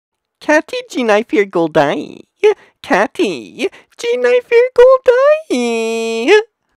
How to Pronounce Kate Jennifer Goldie
Learn the proper way to say and pronounce the name Kate Jennifer Goldie in English. Pronounce and say English words correctly in the Runforthecube voice. English and American pronunciation.